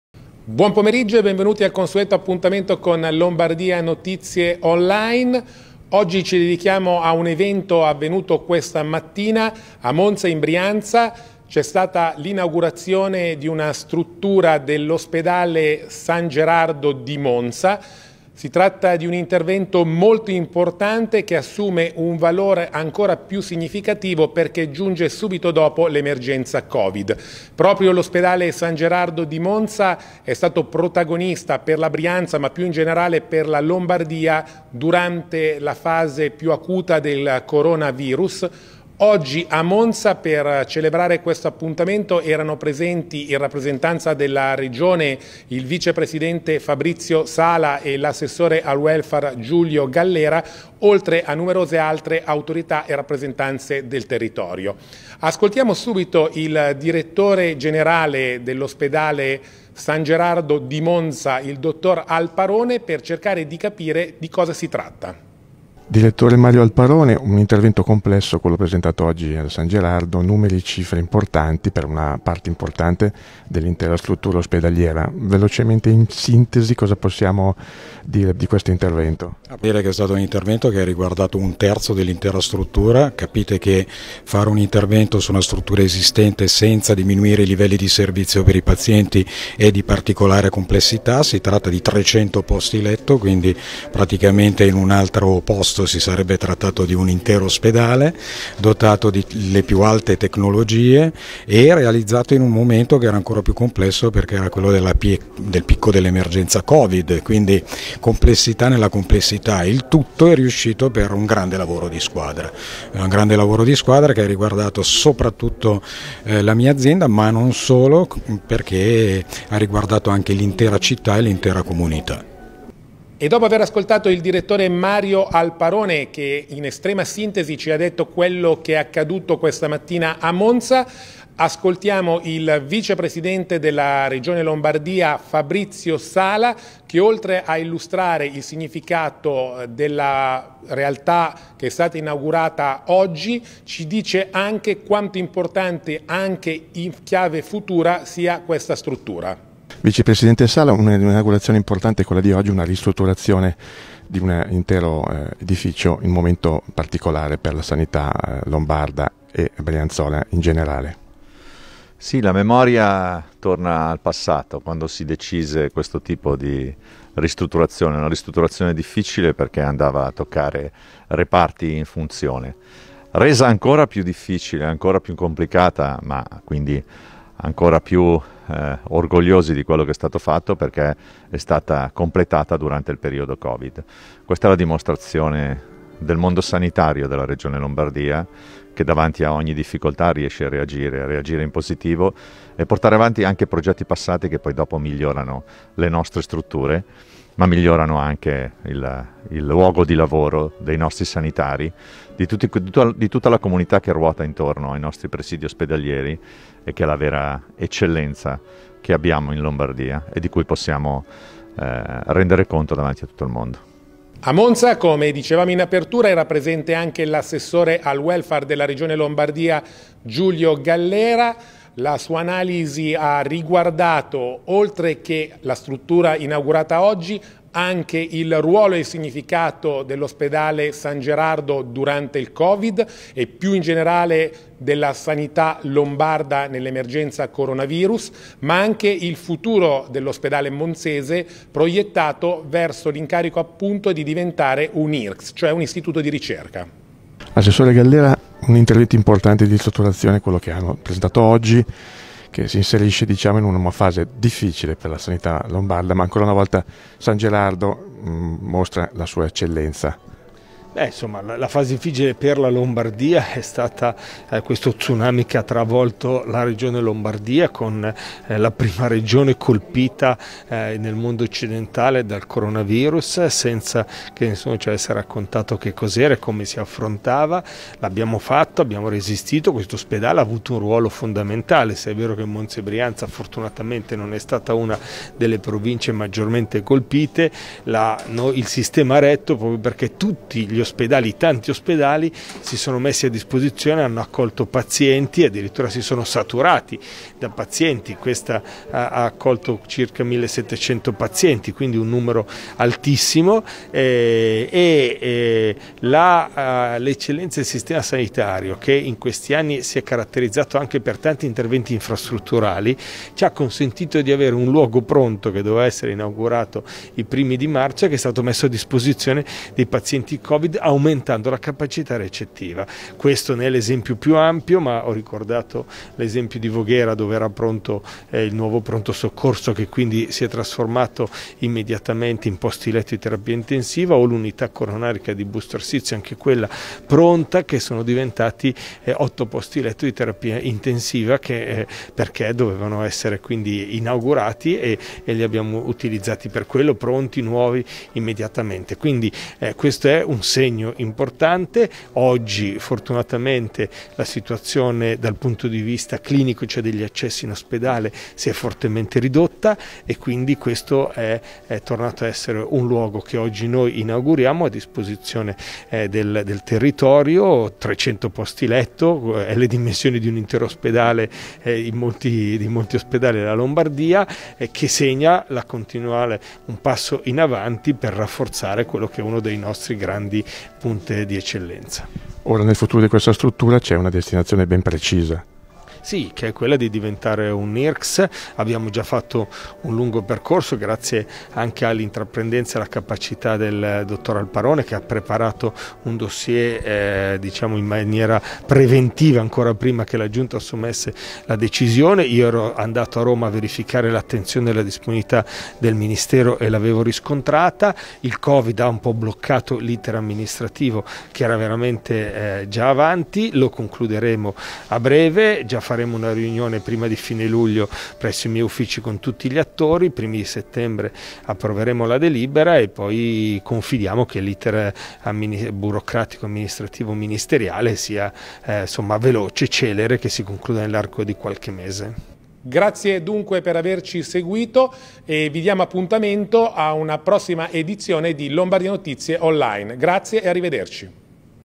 Monza - Inaugurato un settore dell'Ospedale 'San Gerardo' (15.07.20)
- Monza - Inaugurato un settore dell'Ospedale 'San Gerardo'. Fabrizio Sala e Giulio Gallera: "Passo avanti dell'eccellenza regionale. I lavori riguardano anche nuove camere di degenza e interventi di adeguamento energetico". (15.07.20)